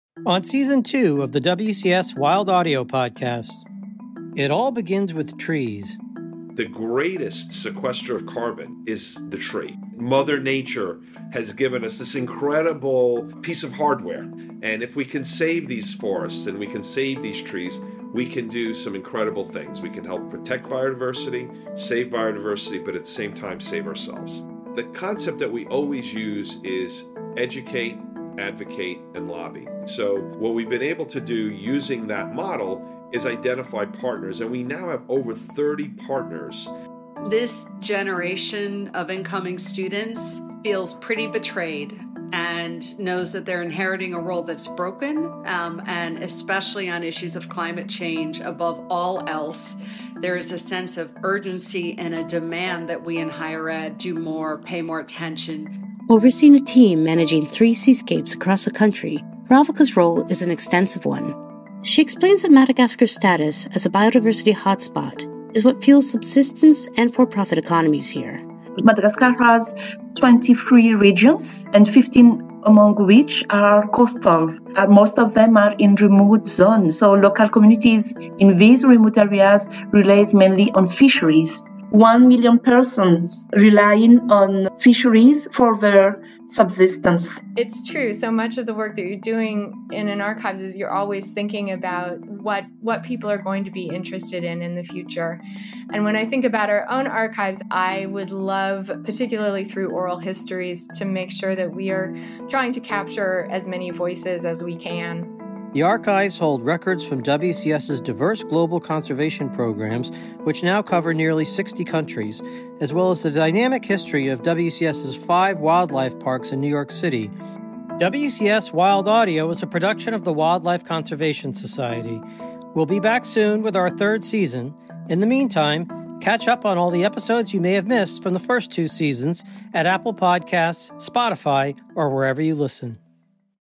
Last Season on WCS Wild Audio
Thanks for joining us for Season Two of WCS Wild Audio. We'll be back soon with new stories from the Wildlife Conservation Society’s field sites, zoos and aquarium, and conservation partners. In the meantime, check out some of the highlights of our conversations with WCS staff from across the world working to protect wildlife and wild places.